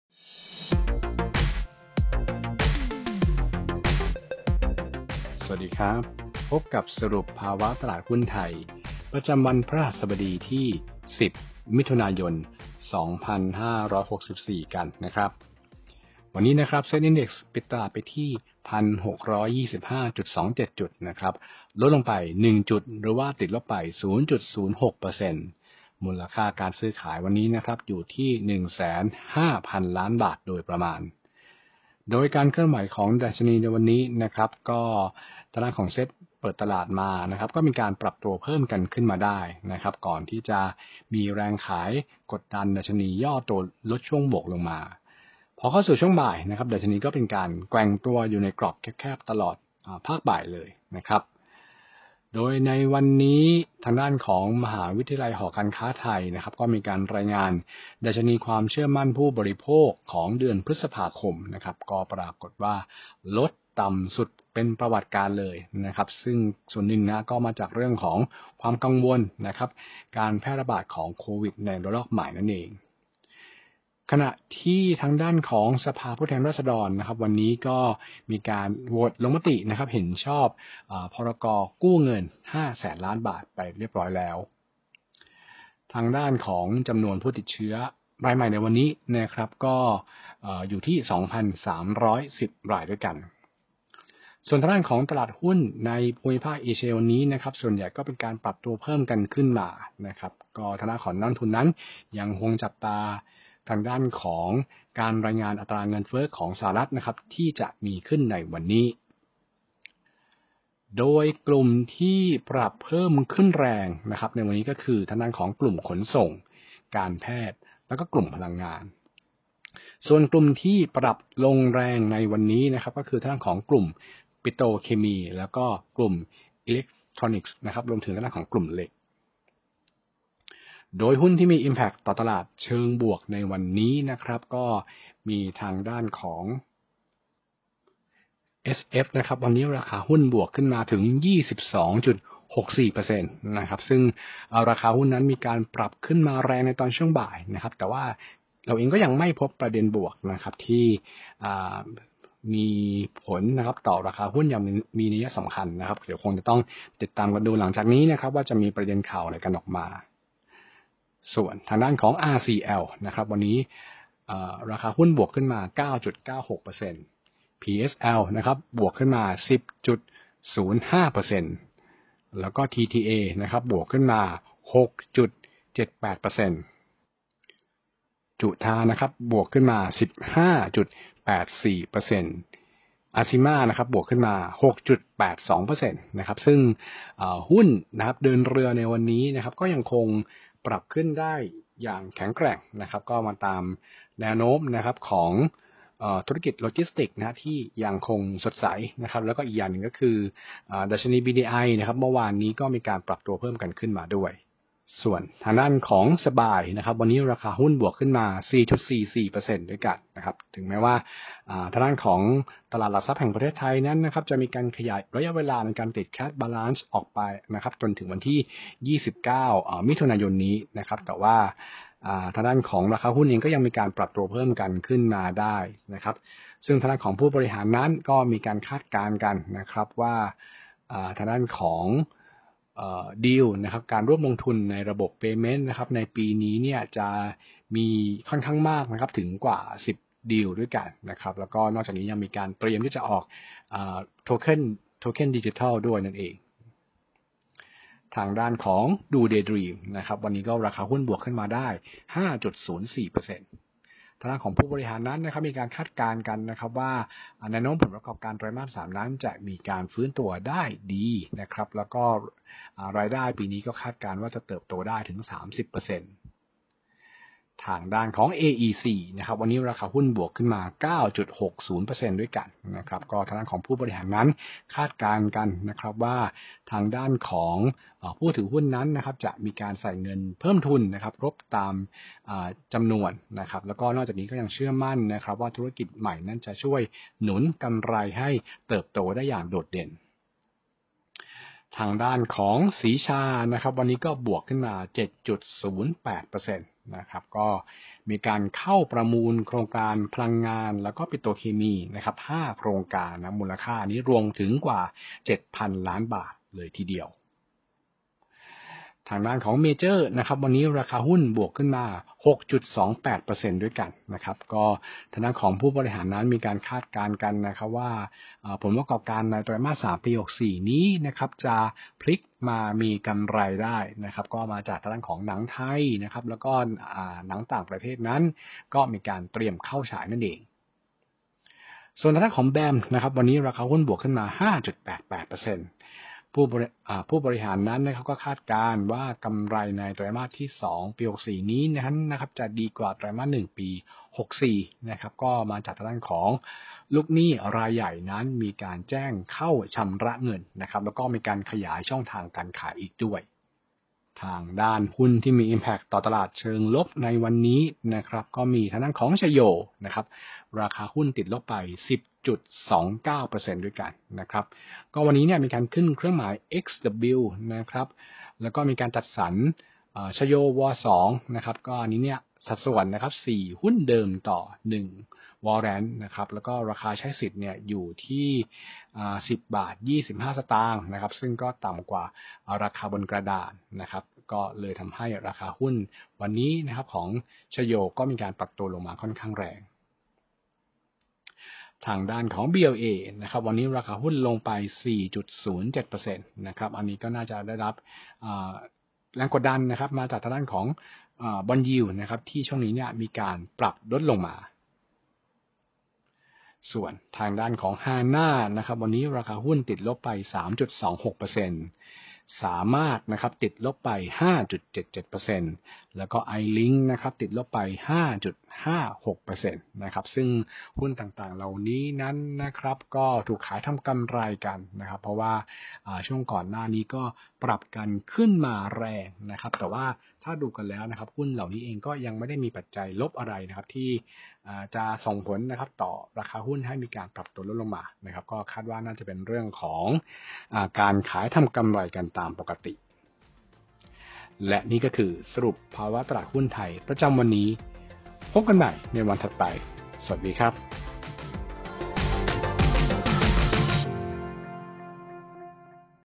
📋 รับฟัง Podcast สรุปภาวะตลาดหุ้นไทย 10 มิ.ย. 2564
📈 SET Index ปิดที่ 1,625.27 จุด (-1.00 จุด/-0.06%) มูลค่าการซื้อขาย 1.05 แสนลบ. SET ปรับเพิ่มขึ้นได้ในช่วงเช้าก่อนจะมีแรงขายลดช่วงบวกลง และแกว่งกรอบแคบตลอดช่วงบ่าย โดย ม.หอการค้าไทย รายงานดัชนีความเชื่อมั่นผู้บริโภค พ.ค. ลดต่ำสุดเป็นประวัติการณ์ จากความกังวลโควิดระลอกใหม่ ขณะที่สภาผู้แทนฯ โหวตเห็นชอบ พ.ร.ก.กู้เงิน 5 แสนลบ. ด้านจำนวนผู้ติดเชื้อรายใหม่วันนี้อยู่ที่ 2,310 ราย ทางด้านตลาดภูมิภาคส่วนใหญ่ปรับเพิ่มขึ้น นลท. ยังคงจับตาการรายงานอัตราเงินเฟ้อสหรัฐวันนี้

💸 กลุ่มที่ปรับเพิ่มขึ้นแรง😄 คือ กลุ่มขนส่ง การแพทย์ พลังงาน ส่วนกลุ่มที่ปรับลดลงแรง😭 คือ กลุ่มปิโตรฯ อิเล็กฯ เหล็ก

👍🏻 หุ้นที่มี impact ต่อตลาดเชิงบวก

RCL (+9.96%) PSL (+10.05%) TTA (+6.78%) JUTHA (+15.84%) ASIMAR (+6.82%) แนวโน้มเดินเรือ-โลจิสติกส์ยังสดใส ดัชนี BDI ปรับขึ้นวานนี้

SABUY (+4.44%) ขึ้นแรงแม้ ตลท. ขยายเวลาติด cash balance ไปถึง 29 มิ.ย. ขณะที่ ผบห.คาดปีนี้มีกว่า 10 ดีลร่วมทุนระบบ Payment เตรียมออกเหรียญดิจิทัล

DDD (+5.04%) ผบห.คาดไตรมาส 3 ฟื้นตัว คงเป้ารายได้ปีนี้โต 30%

AEC (+9.60%) ผบห.คาดผู้ถือหุ้นเดิมใส่เงินเพิ่มทุนครบ เชื่อมั่นธุรกิจใหม่หนุนกำไรเติบโตเด่น

SRICHA (+7.08%) เข้าประมูลโครงการพลังงานและปิโตรเคมี 5 โครงการ มูลค่ารวม 7 พัน ลบ.

MAJOR (+6.28%) ผบห.คาดว่า 3Q64 พลิกมามีกำไรจากหนังไทย- ตปท.เตรียมเข้าฉาย

BAM (+5.88%) ผบห.คาดกำไรสุทธิ Q2/64 ดีกว่า Q1/64 หลังลูกหนี้รายใหญ่แจ้งชำระเงิน-ขยายช่องทางขาย

👎🏻 หุ้นที่มี impact ต่อตลาดเชิงลบ

CHAYO (-10.29%) วันนี้ขึ้นเครื่องหมาย XW จัดสรร CHAYO-W2 ในสัดส่วน 4 หุ้นเดิมต่อ 1 ใบสำคัญแสดงสิทธิ ราคาใช้สิทธิต่ำกว่ากระดาน10.25 บาท อายุ 2 ปี

BLA (-4.07%) bond yield ระยะนี้ปรับลดลง

HANA (-3.26%) SAMART (-5.77%) ILINK (-5.56%) ถูกขายทำกำไรหลังจากช่วงก่อนหน้าปรับขึ้นแรง